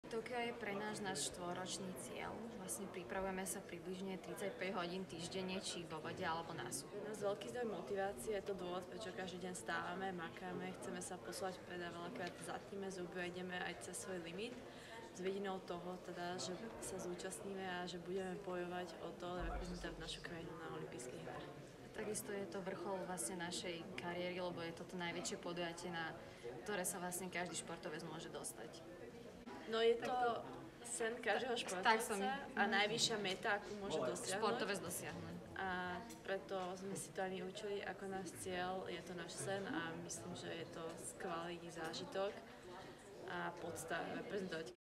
Tokyo 2020: Nada Daabousová a Diana Miškechová
Nada s Dianou nám řekli co pro ně znamená když se řekne Tokyo 2020